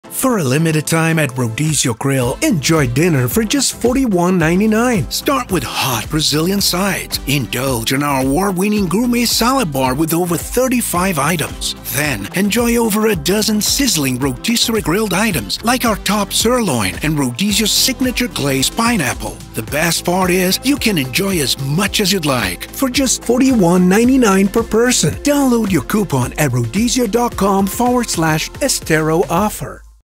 Rodizio Grill Coconut Point Special Offer - Full Rodizio Dinner Only $41.99/person.
Unlimited Grilled Meats, Brazilian Salads and sides for only $41.99. Hurry This Offer Ends July 31, 2024.